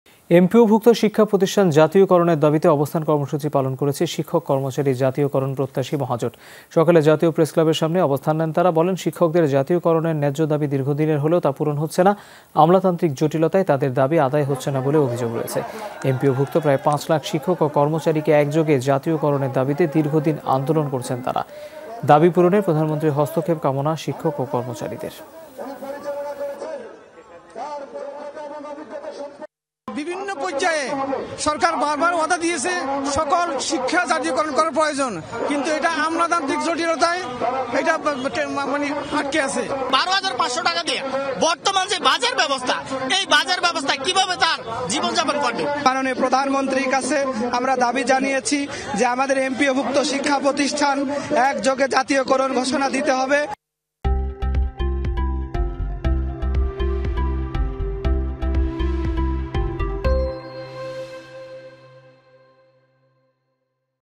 জাতীয়করণের দাবিতে প্রেস ক্লাবের সামনে শিক্ষকদের অবস্থান | Teacher Protest
এমপিওভুক্ত শিক্ষা প্রতিষ্ঠান জাতীয়করণের দাবিতে অবস্থান কর্মসূচী পালন করেছে শিক্ষক কর্মচারী জাতীয়করণ প্রত্যাশী মহাজোট। সকালে জাতীয় প্রেসক্লাবের সামনে অবস্থান নেন তারা। বলেন, শিক্ষকদের জাতীয়করণের ন্যায্য দাবি দীর্ঘদিনের হলেও তা পূরণ হচ্ছে না। আমলাতান্ত্রিক জটিলতায় তাদের দাবি আদায় হচ্ছে না বলেও অভিযোগ তাদের। এমপিওভুক্ত প্রায় ৫ লাখ শিক্ষক ও কর্মচারীকে একযোগে জাতীয়করণের দাবিতে দীর্ঘদিন আন্দোলন করছেন তারা।  বলেন, শিক্ষাব্যবস্থার ৯৭ শতাংশ হলেও, এমপিও শিক্ষক কর্মচারীরা বৈষম্যের স্বীকার। দাবি পূরণে প্রধানমন্ত্রীর হস্তক্ষেপ কামনা করেন শিক্ষক ও কর্মচারীরা।